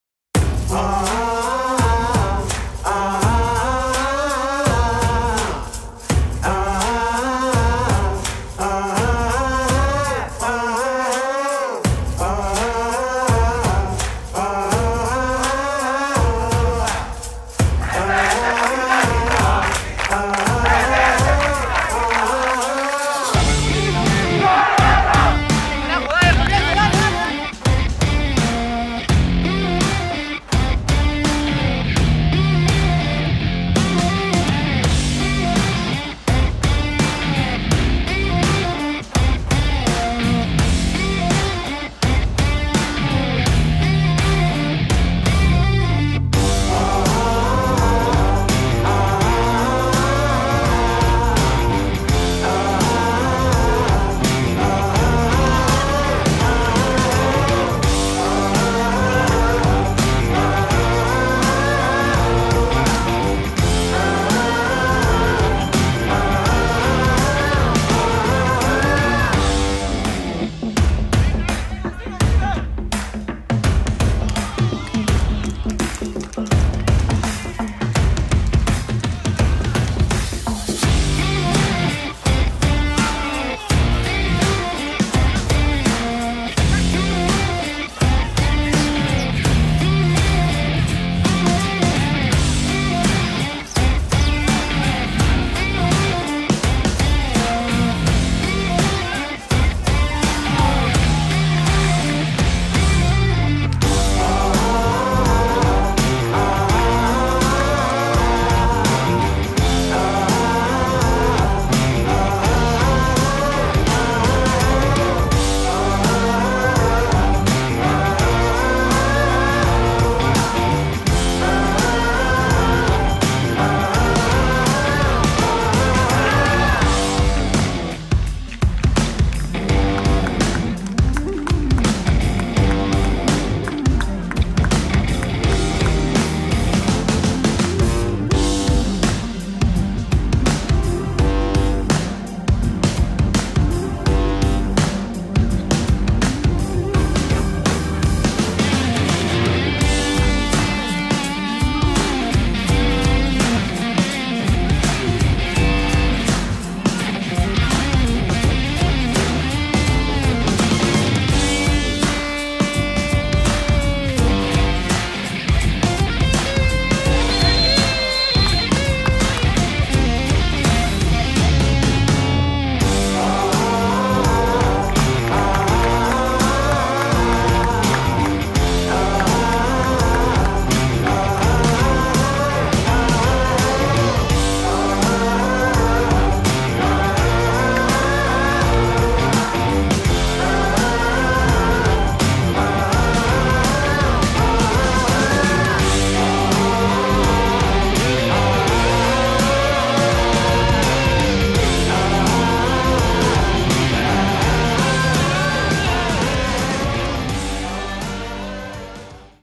Desde otro ángulo: EL TRIUNFO de la Sub-18 ante Comerciantes Unidos l Cristal TV
El equipo de Jorge Cazulo sumó su sexto triunfo consecutivo al vencer por  6-1 a Comerciantes Unidos en La Florida por la Copa Generación. Los goles, las jugadas y mucho más aquí en nuestro Canal Oficial.

Fecha 7
La Florida, 1 de noviembre de 2021
Imágenes: Cristal TV